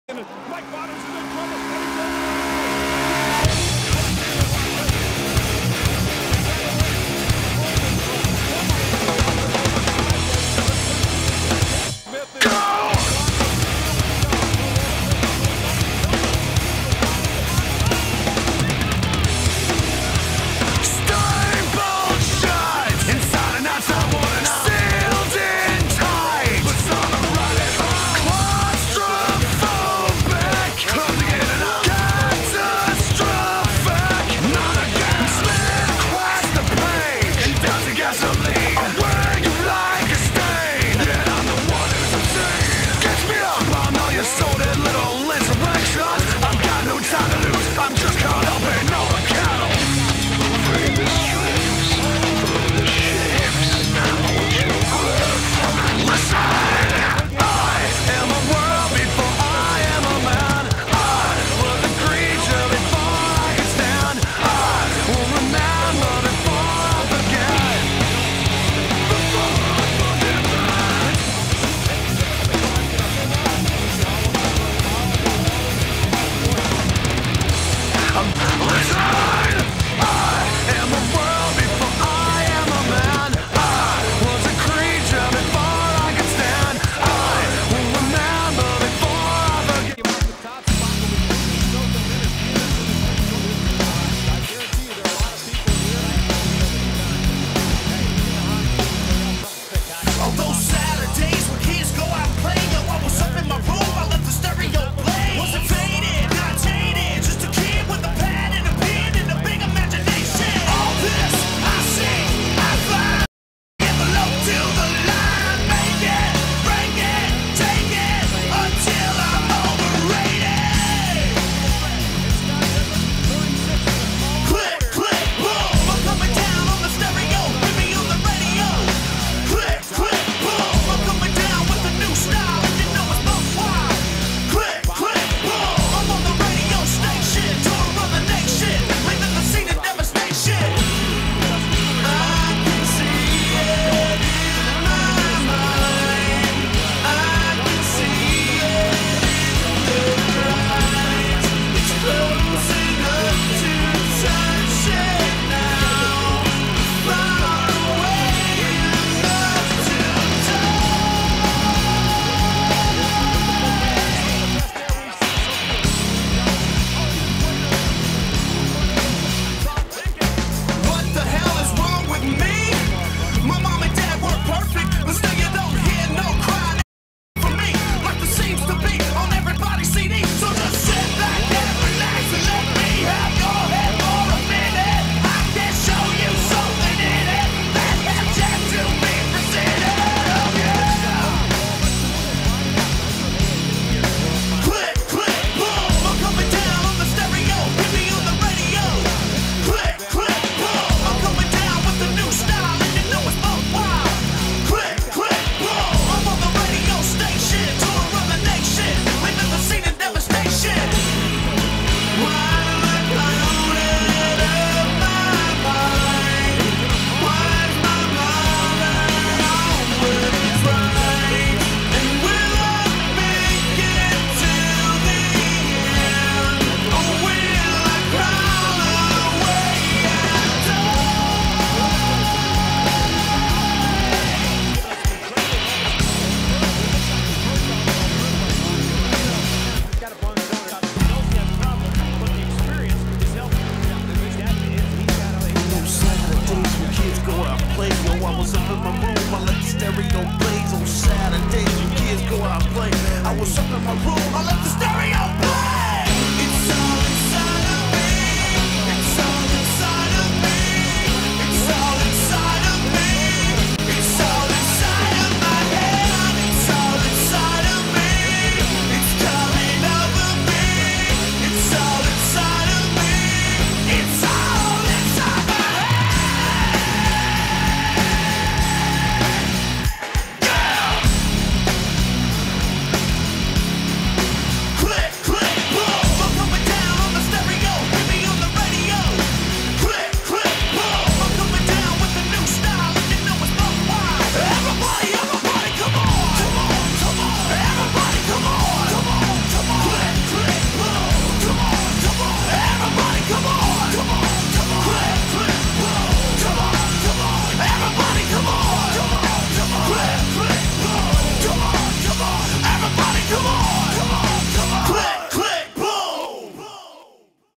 World Finals 4 Highlights!
FUN FACT! This world finals features the least number of crashes in racing and freestyle combined, with a total of 5. One in racing from Mike Vaters senior, and four in freestyle, being Monster Mutt, Lyle Hancock in Blue Thunder, Dennis Anderson in Grave Digger, and Tom Meents with the first ever world finals hit of Maximum Destruction. There're 6 if you count Blue Thunder's encore rollover.